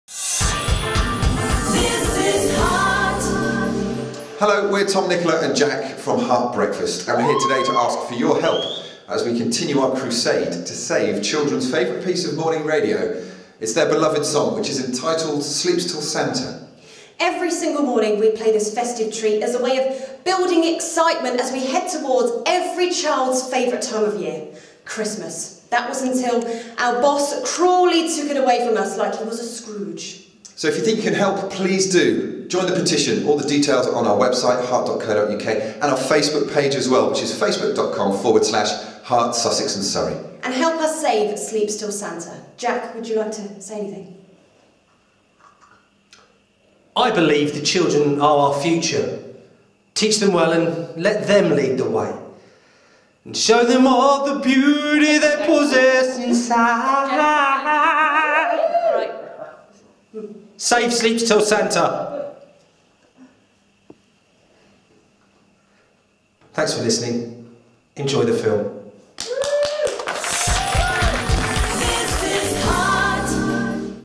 Save Sleeps Til Santa at Cineworld Brighton
After Heart's scrooge-like boss announced that Heart Breakfast's much-loved pre-Christmas institution isn't going to return this December, Tom, Nicola and Jack have taken upon themselves to launch a campaign to bring back 'Sleeps Til Santa'. Thanks to Cineworld Brighton for backing the campaign and running this video message before a screening of The Hunger Games 2.